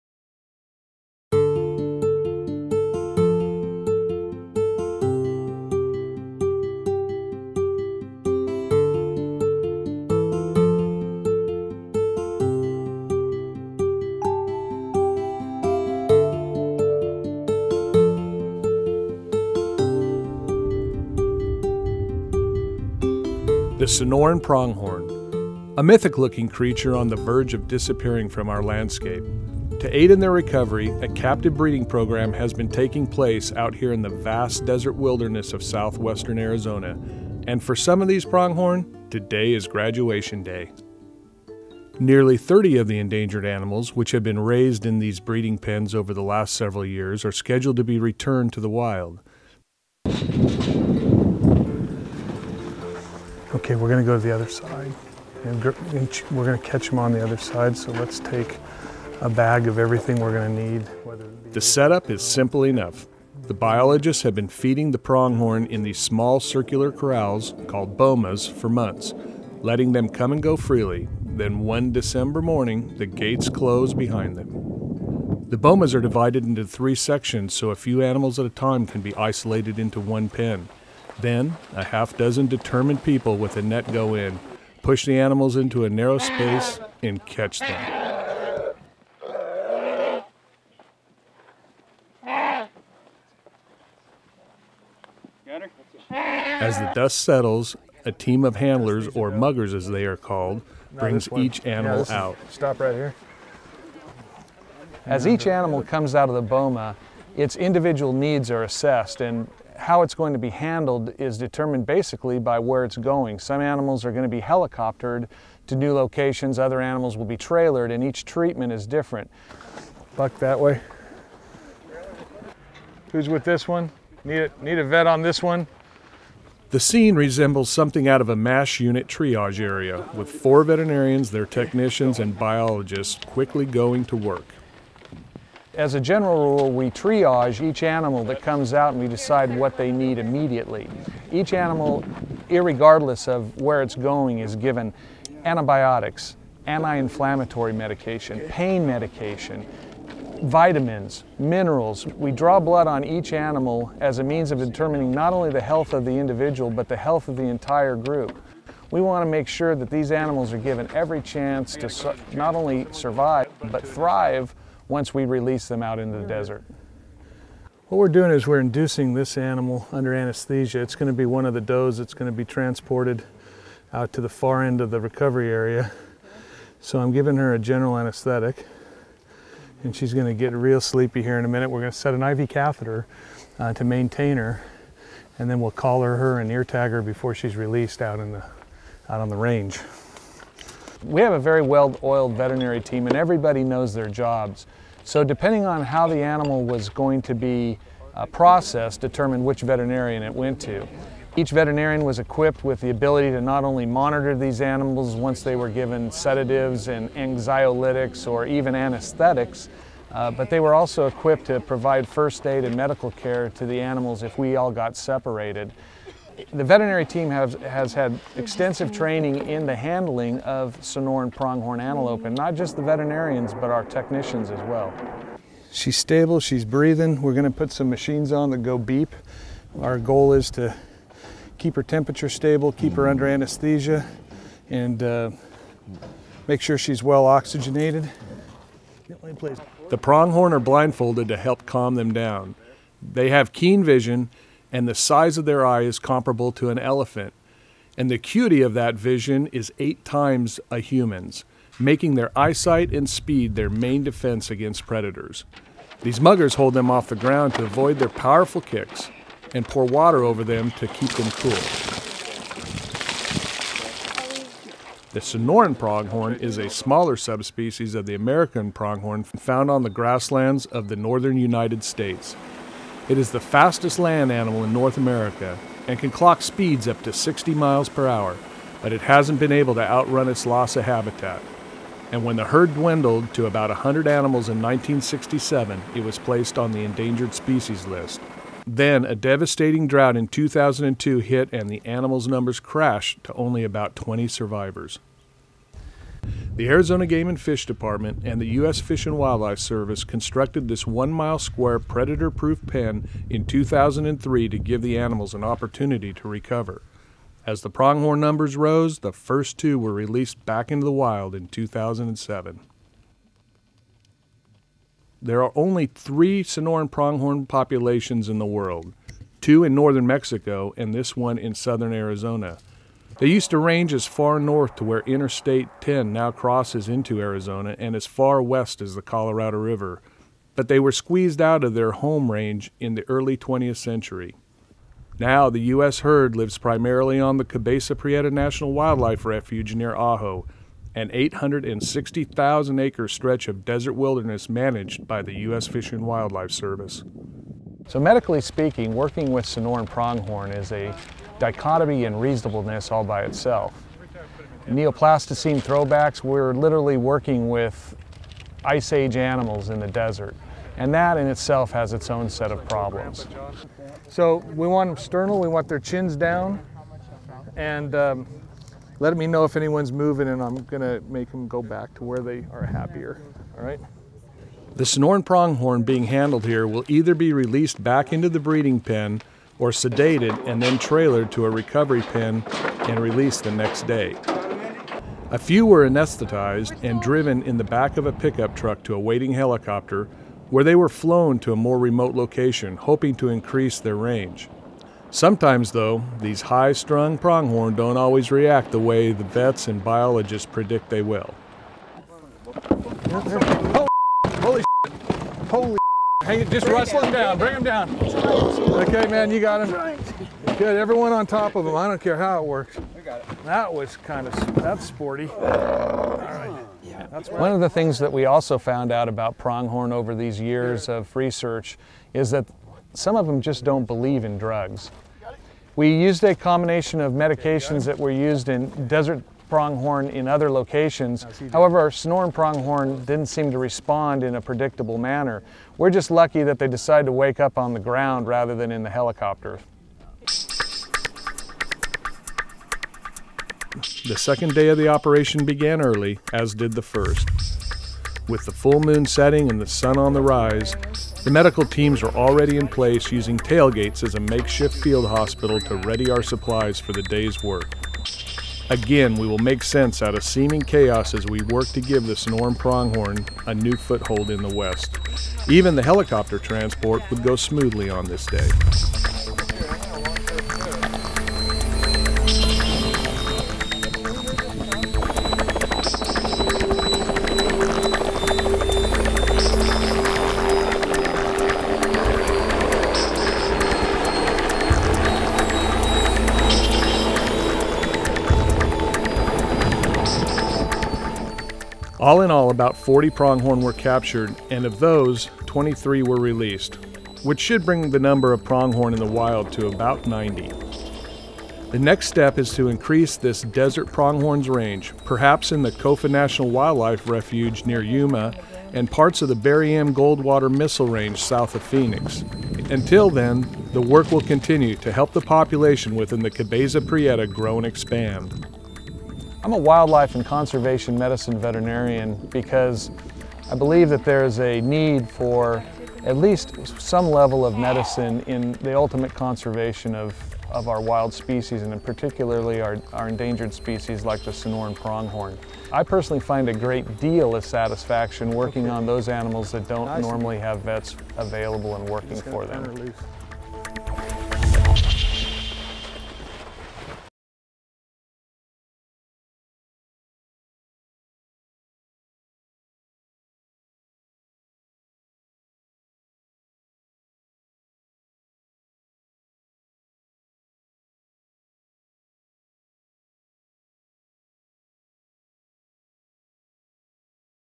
Sonoran Pronghorn .mov
Endangered Sonoran Pronghorn are being raised in captivity and released back into the wild as their numbers permit.